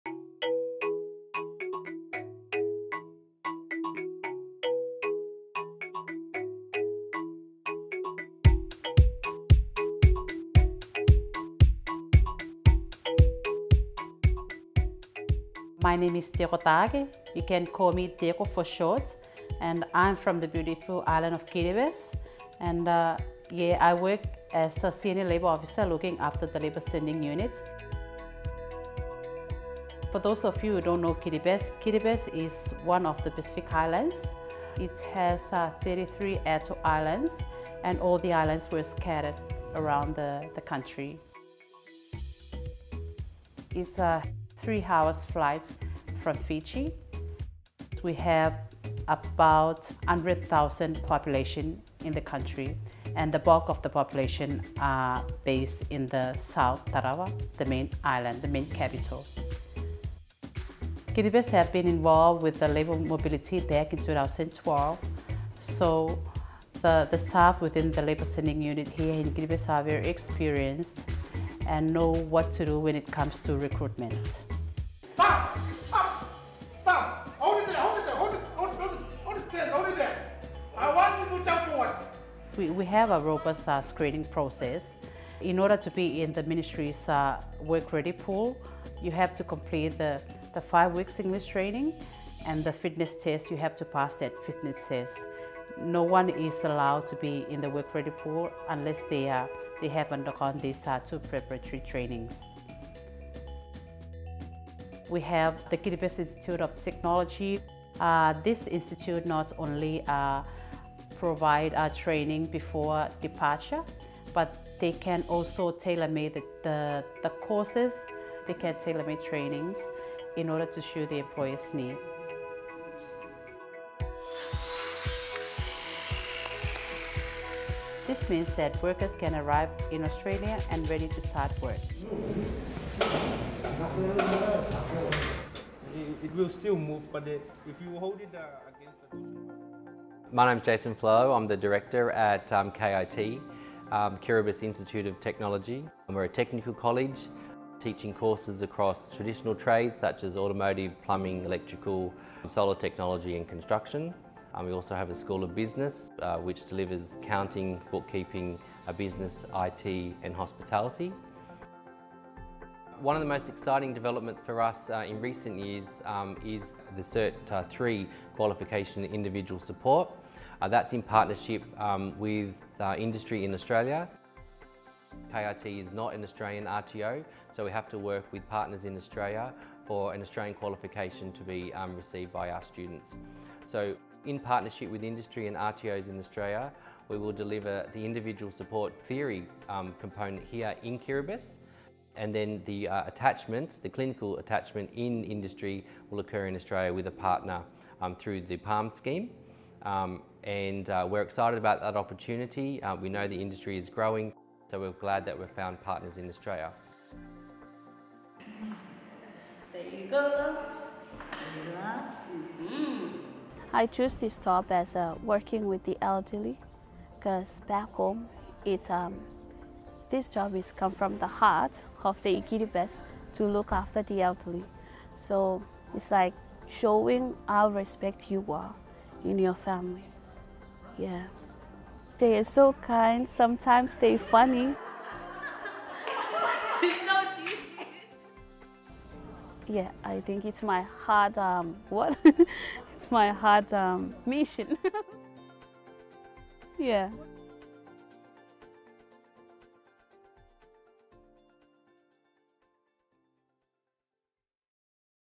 'Mauri' from Kiribati
Kiribati is made up of 33 atolls, spread over 3.5 million km² of ocean – an area the size of Western Australia and South Australia combined. Kiribati has been involved in labour mobility since 2012, sending workers to Australia for the aged care, meat processing, horticulture and tourism sectors. Learn more about this beautiful country and it's people, in this short video.